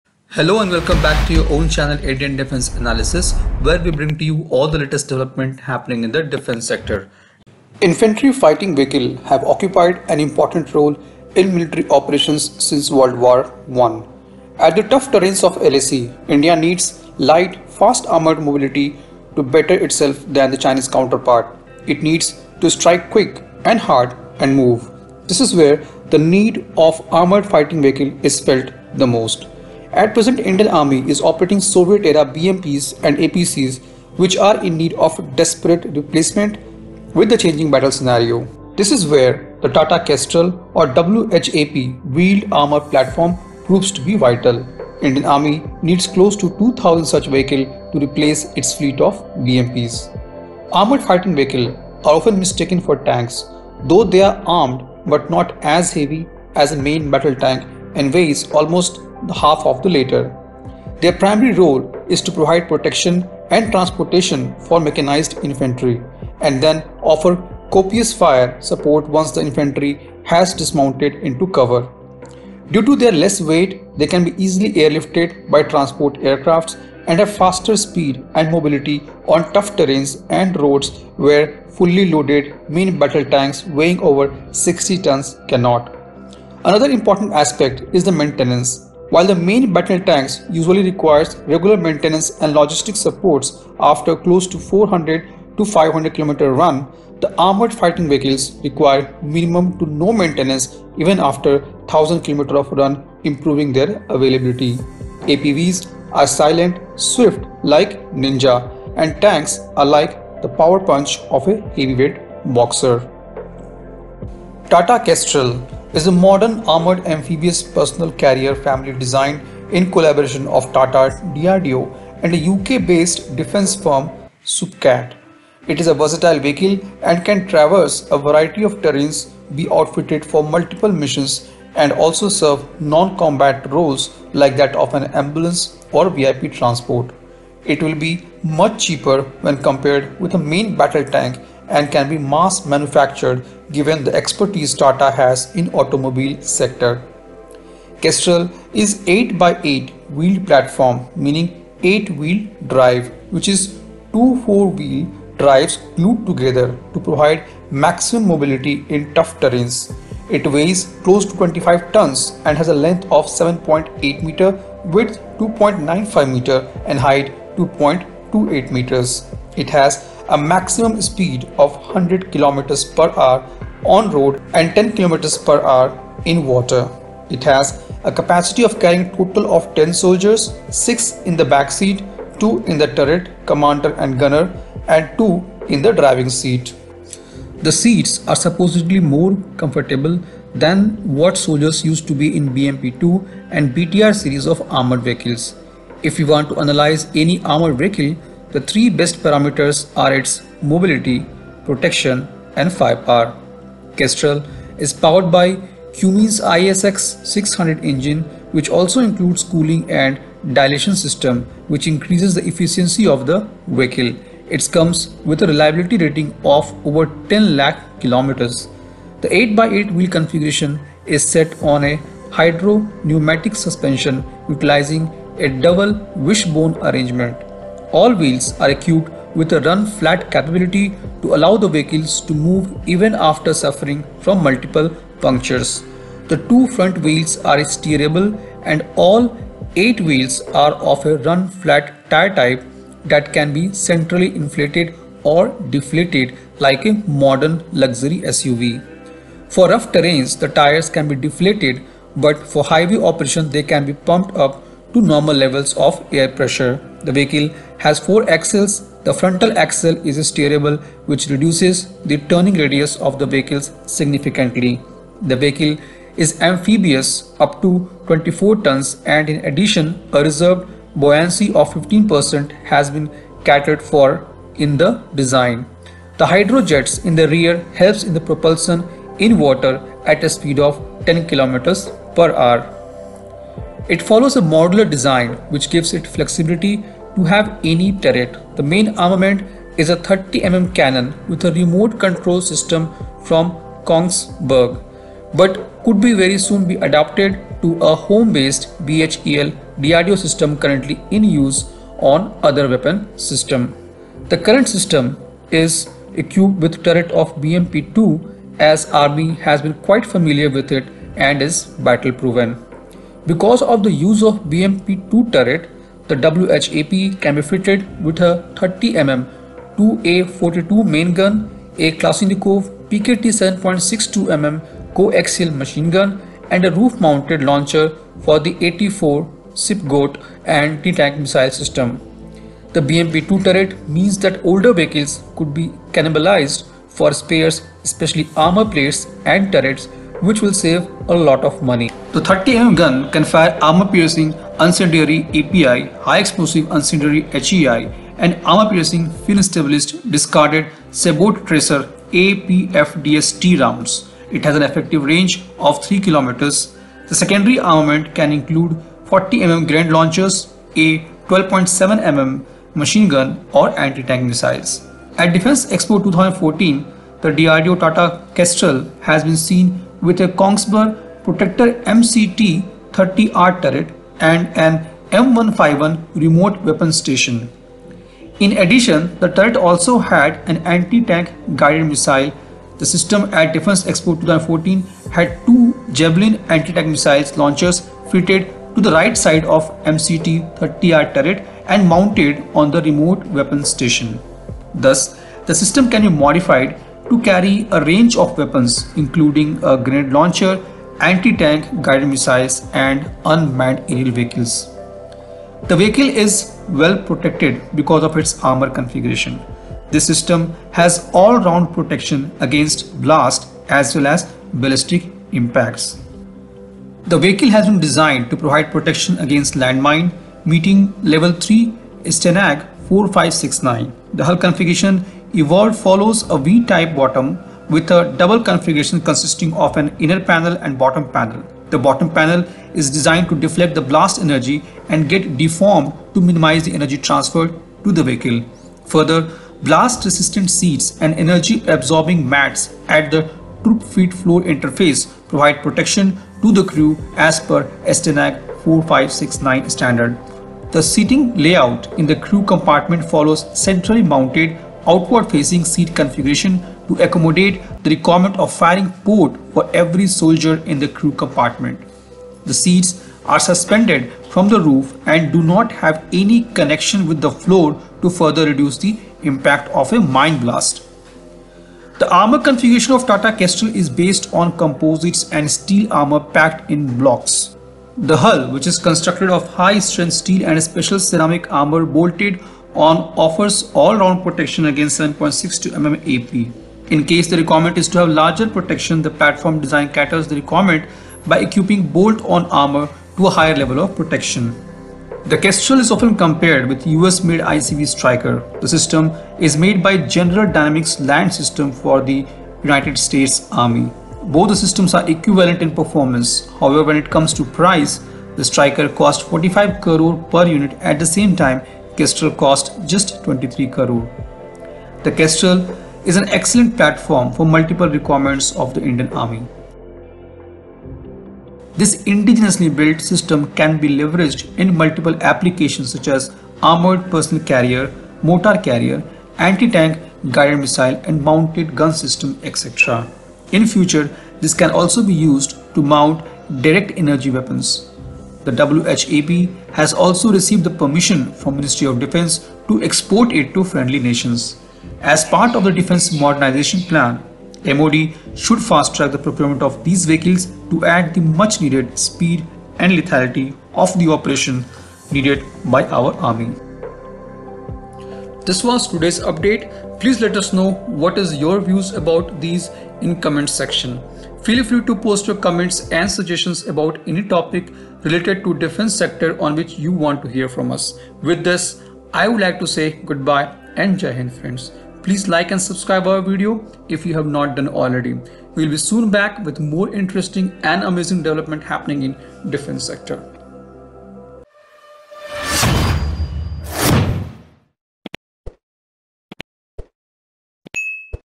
DRDO TATA Kestrel 8x8 Wheeled Armored Amphibious Platform (WhAP / WAP)
Infantry fighting vehicles have occupied an important role in military operations since World War I. At the tough terrains of LAC, Indian needs light, fast armored mobility to better itself than the Chinese counterpart. It needs to strike quick & hard and move. This is where the need to Armored Fighting vehicle is felt the most. At present Indian Army is operating the Soviet Era BMPs and APCs which are in need of desperate replacement with the changing battle scenario. This is where the TATA Kestrel or WhAP(Wheeled Armoured Platform) proves to be vital. Indian Army needs close to 2000 such vehicle to replace its fleet of BMPs. Armored Fighting Vehicles are often mistaken for tanks. Though they are armed but not as heavy as a main battle tank and weighs almost of half the later. Their primary role is to provide protection and transportation for mechanized infantry —and then offer copious fire support once the infantry has dismounted into cover. Due to their less weight, they can be easily airlifted by transport aircrafts and have faster speed and mobility on tough terrains and roads where the fully loaded main battle tanks weighing over 60 tons cannot. Another important aspect is the maintenance. While the Main battle tanks usually requires maintenance and logistics support after close to 500KM run, the Armored Fighting vehicles require minimum to no maintenance even after 1000KM of run improving their availability. APVs are silent and swift like Ninja and Tanks are the like the power punch of a heavy weight boxer. The TATA Kestrel or WhAP(Wheeled Armoured Platform) is a modern armoured amphibious personnel carrier family designed in a collaboration of TATA, DRDO, and a UK based defence firm, Supacat. It is a versatile vehicle and can traverse a variety of terrains, be outfitted for multiple missions and also serve non-combat roles like that of an ambulance or VIP transport. It will be much, cheaper when compared with a main-battle tank and can be mass-manufactured given the expertise Tata has in automobile sector. Kestrel is an 8×8 wheeled platform, meaning 8-wheel drive, which is two four-wheel-drives glued together, to provide maximum mobility in tough terrains It weighs close to 25 Tonnes and has Length of 7.80m, Width 2.95m and Height 2.28 m. It has a max Speed of 100 KM/H on-road and 10 KM/H in water. It has a capacity of carrying a total of 10 soldiers, 6 in the back seat, 2 in the turret (commander and gunner), and 2 in the driving seat. The seats are supposedly more comfortable than what soldiers were used to in the BMP-2 and BTR series of armoured vehicles. If we want to analyze any armored vehicle the three best parameters are its: Mobility, Protection & Firepower. Kestrel is powered by Cummins ISX 600 engine which also includes cooling and a dilatation system which increases the efficiency of the vehicle. It comes with a reliability rating of over 10 lakh kilometers. The 8X8 wheel configuration is set on a hydropneumatic suspension utilizing a double-wishbone arrangement.  All wheels are equipped with a “run-flat” capability to allow the vehicle to move even after suffering from multiple punctures. The two front wheels are steerable, and all eight wheels are of a run-flat type that can be centrally inflated or deflated, like in a modern luxury SUV. For rough terrains, the tires can be deflated, but for highway operation, they can be pumped up to normal levels of air pressure. The vehicle has four axles, the front axle is steerable which reduces the turning radius of the vehicle significantly. The vehicle is amphibious up to 24 tons and in addition, a reserved buoyancy of 15% has been catered for in the design. Hydro-jets in the rear helps in propulsion in the water at a speed of 10KM/H. It follows a modular design which gives it flexibility to have any turret.